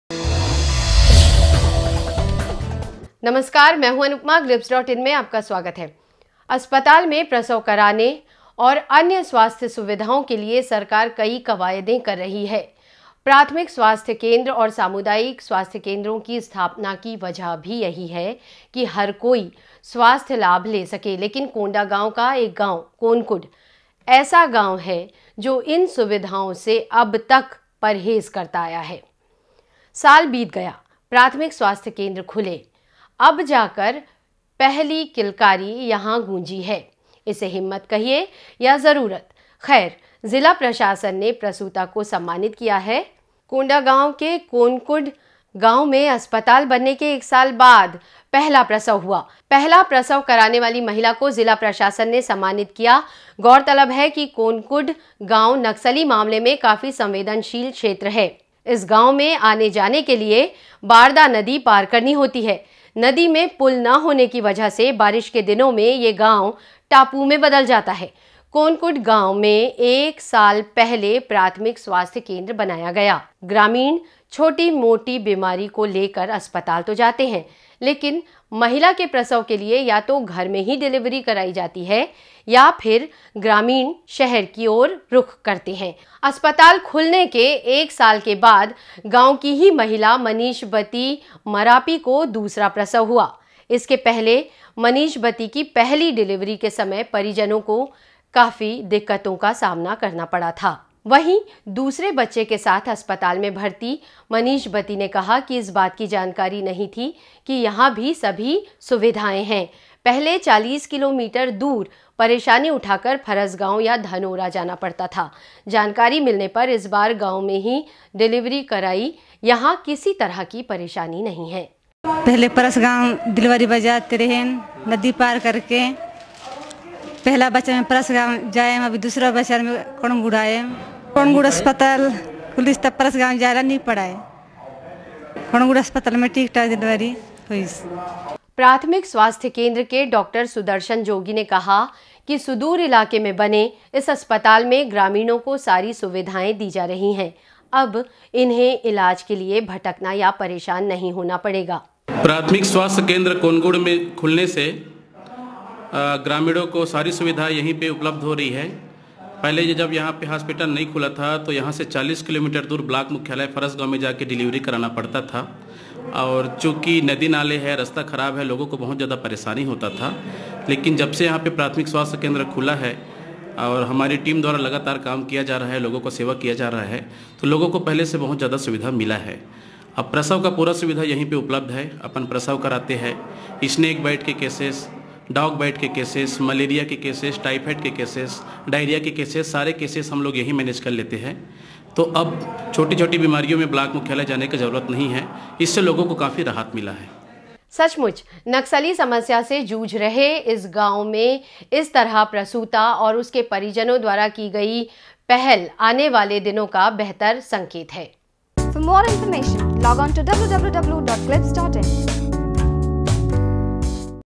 Exclusive: स्थापना के साल भर बाद गूंजी पहली किलकारी
अस्पताल में प्रसव कराने और अन्य स्वास्थ्य सुविधाओं के लिए सरकार कई कवायदें कर रही है प्राथमिक स्वास्थ्य केंद्र और सामुदायिक केन्द्रों की स्थापना की वजह भी यही है की हर कोई स्वास्थ्य लाभ ले सके लेकिन कोंडागांव का एक गाँव कोनकुड ऐसा गाँव है जो इन सुविधाओं से अब तक परहेज करता आया है साल बीत गया प्राथमिक स्वास्थ्य केंद्र खुले अब जाकर पहली डिलीवरी यहाँ हुई है .इसे हिम्मत कहिये या ज़रुरत खैर जिला प्रशासन ने प्रसूता को सम्मनित किया है To read more log